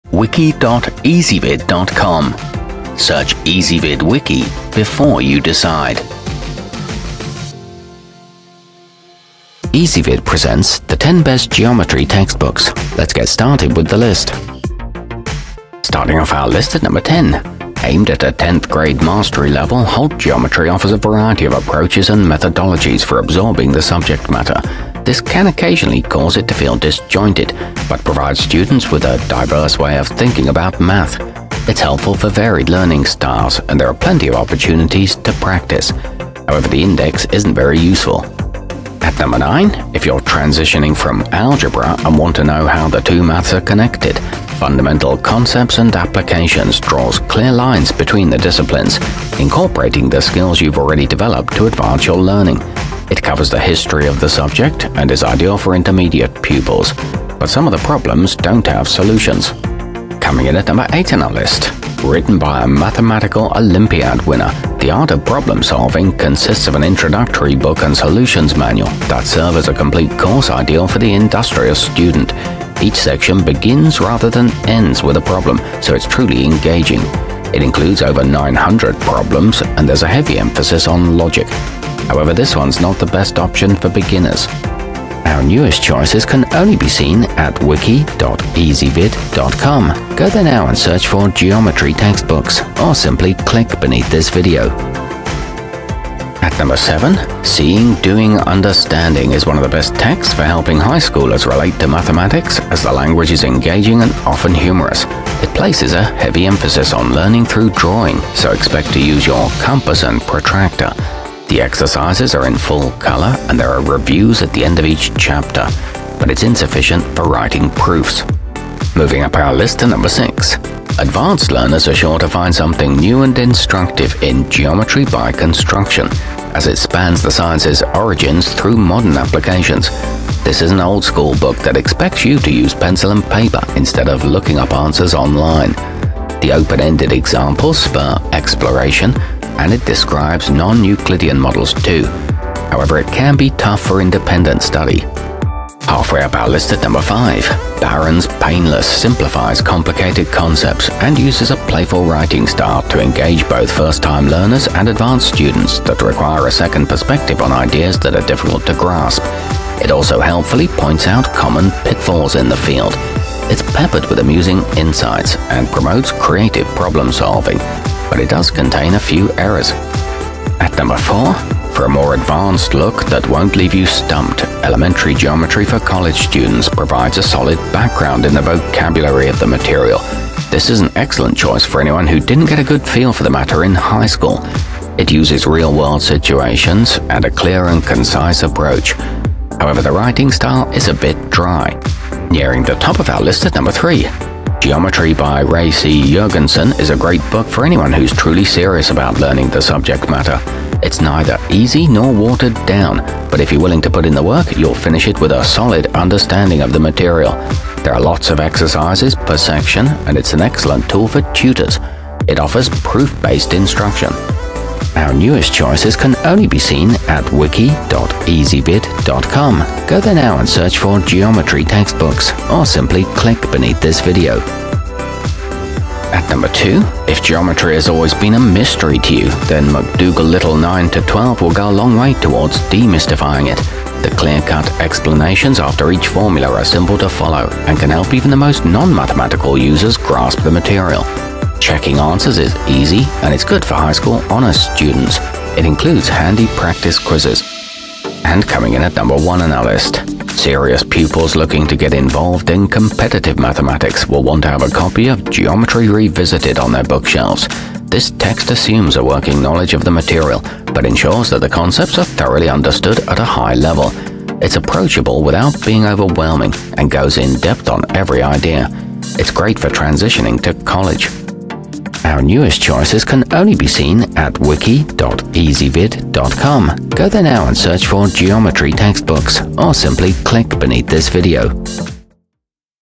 10 Best Geometry Textbooks 2018
Our complete review, including our selection for the year's best geometry textbook, is exclusively available on Ezvid Wiki.

Geometry textbooks included in this wiki include the seeing, doing, understanding, geometry revisited, mcdougal littell 9-12, geometry by ray c. jurgensen, the art of problem solving, elementary geometry for college students, holt geometry, geometry by construction, fundamental concepts and applications, and barron's painless.